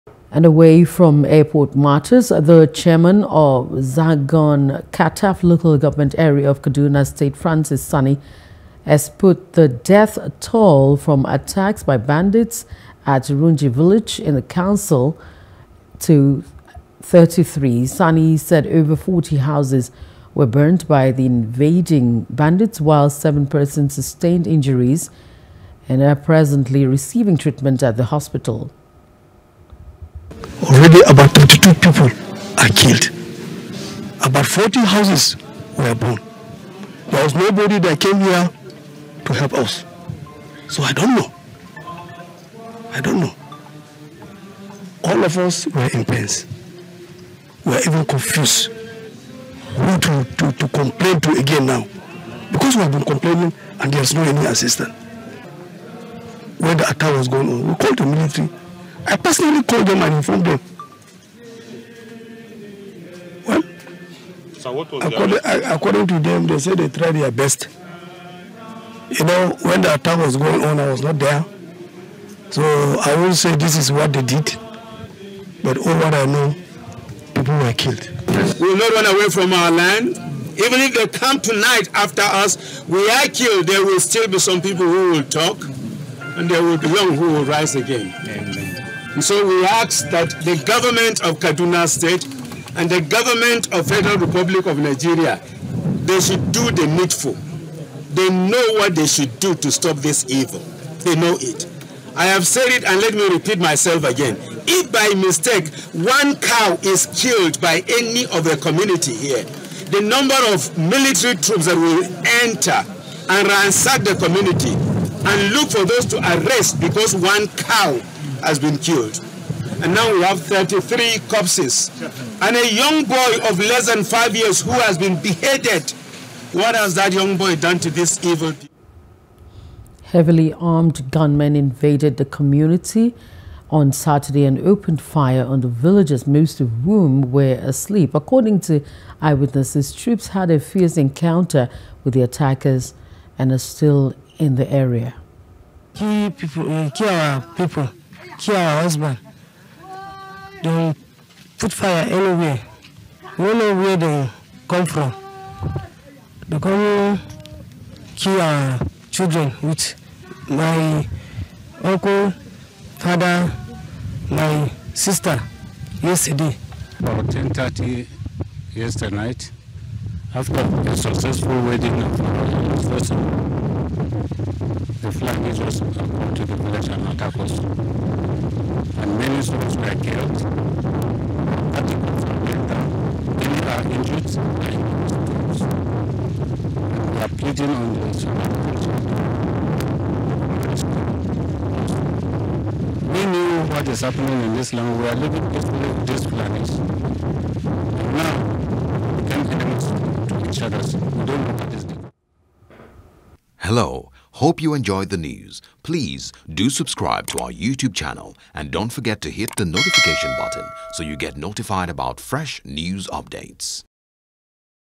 Banditry: Death Toll In Southern Kaduna Killings Rises To 33, Victims Get Mass Burial
The Chairman of Zangon Kataf Local Government Area of Kaduna State, Francis Sani, has put the death toll from attacks by bandits at Runji Village in the council on Saturday at 33.